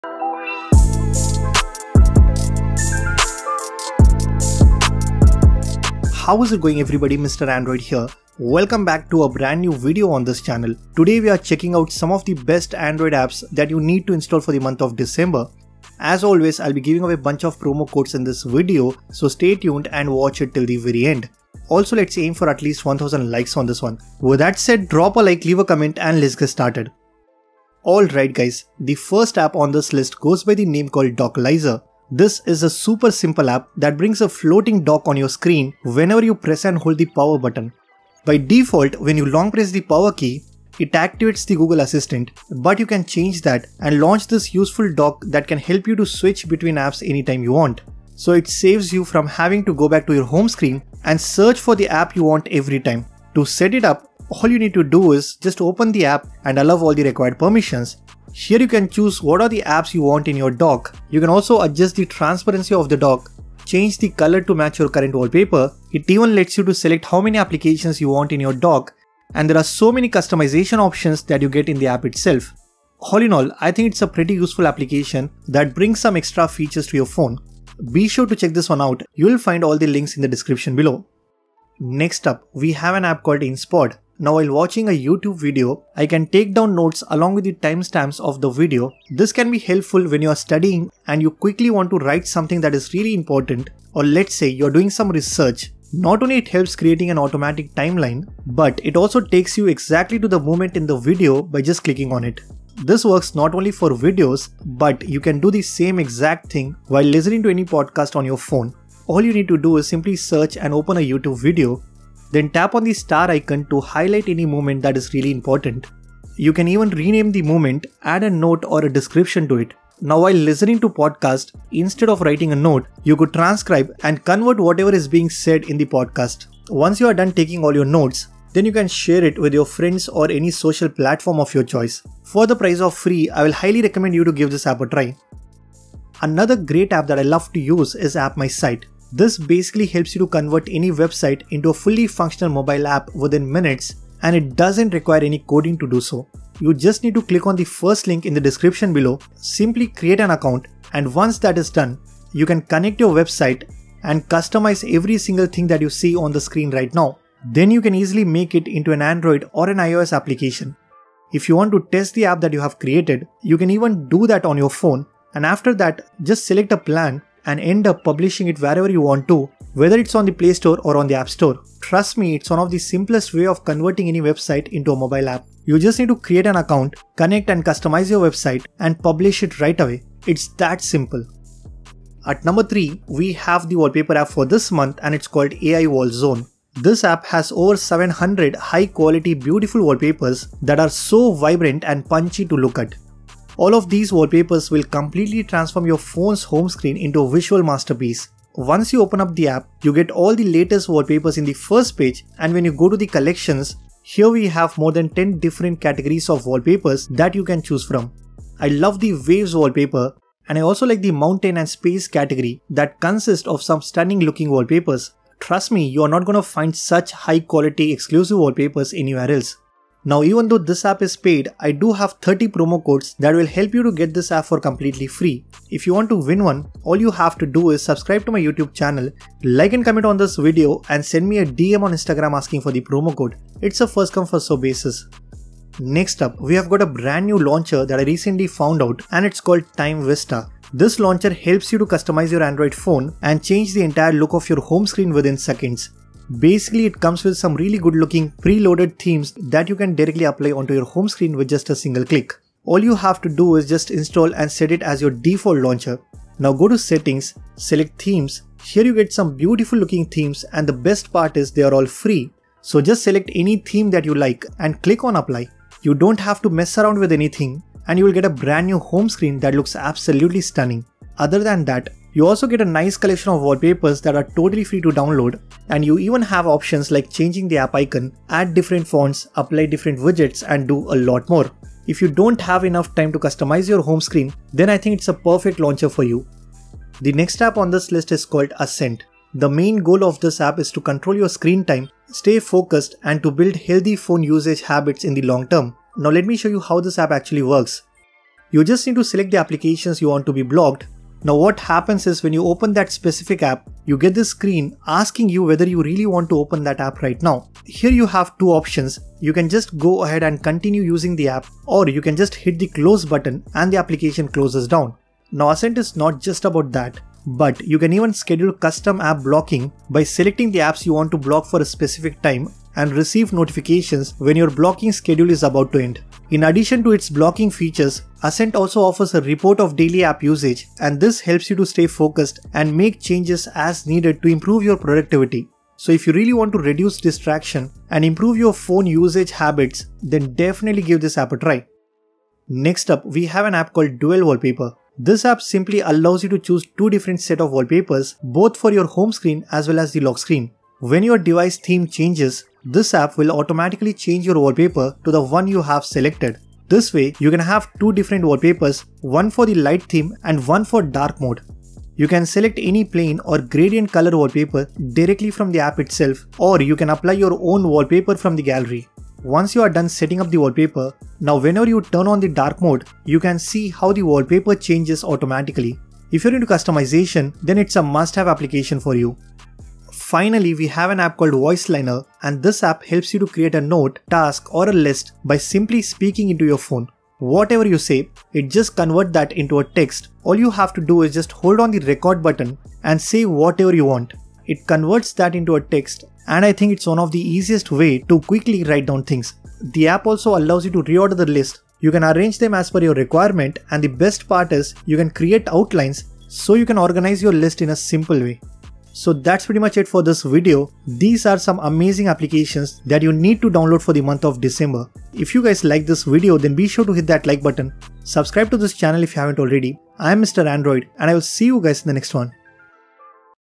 7 SUPERIOR Android Apps You Need To INSTALL Right NOW - December 2023!
7 SUPERIOR Android Apps You Need To INSTALL Right NOW - December 2023!

In this video, I'll be showing you 7 Superior Android apps that you need to install on your android device right now.

Follow me on Threads for some exclusive tech content and leaks

Timestamps :
00:00 intro
00:27 1st App
01:22 2nd App
02:16 AppMySite (Sponsor)
03:02 3rd App
03:56 4th App
04:47 5th App
05:43 6th App
06:20 7th App
06:50 outro

Background music